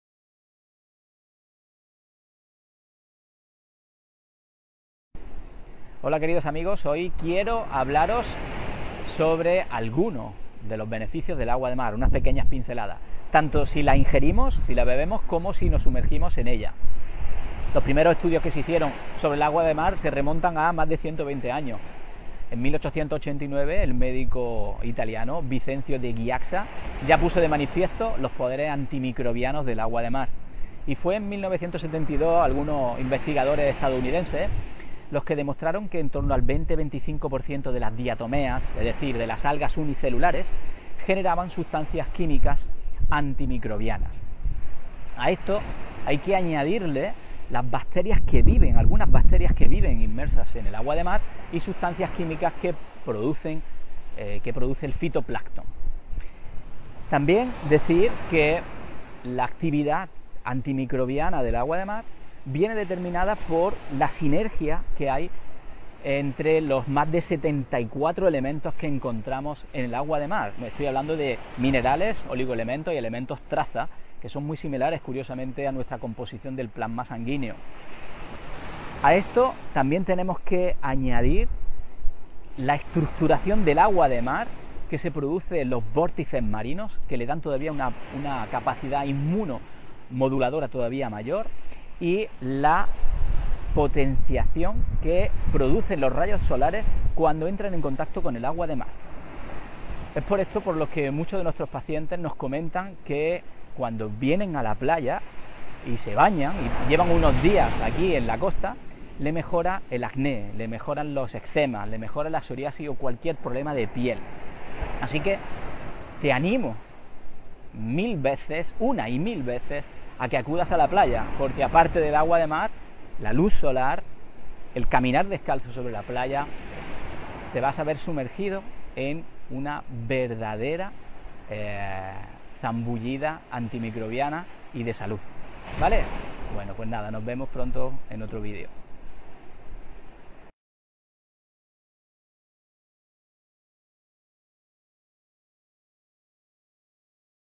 Sumérgete en la fuente de la vida: ¡el agua de mar!
En ME TÓ DI CA, estamos fascinados por los innumerables beneficios que el agua de mar puede aportar a nuestra salud y bienestar. Ya sea que lo consumamos o nos sumerjamos en él, el océano es una fuente inagotable de vitalidad.

Beneficios de beber agua de mar:
¿Sabías que el agua de mar es una rica fuente de minerales y oligoelementos esenciales? Al tomar pequeñas cantidades diariamente, puedes ayudar a equilibrar los niveles de electrolitos, mejorar la digestión y fortalecer el sistema inmunológico. ¡Un elixir natural para revitalizar nuestro cuerpo!

Sustancias antimicrobianas naturales:
El 20-25% de las diatomeas marinas generan poderosas sustancias químicas antimicrobianas. Estas maravillas naturales ayudan a combatir patógenos dañinos y a mantener tu cuerpo en su mejor forma!

Baños en el mar para el alma:
Sumergirse en el océano no solo es refrescante, sino también terapéutico. El agua salada es conocida por su capacidad para aliviar el estrés, reducir la inflamación y promover una piel radiante.

¿Quieres experimentar los beneficios del agua de mar de manera segura y controlada? Ven a nuestra clínica y descubre cómo incorporar estos maravillosos regalos del océano en tu rutina diaria. Os orientamos y os proporcionamos opciones personalizadas para maximizar vuestros resultados.

¡No esperes más para sumergirte en la salud y la vitalidad que el agua de mar tiene para ofrecernos!

📍C/ Pintora Maruja Mallo, 3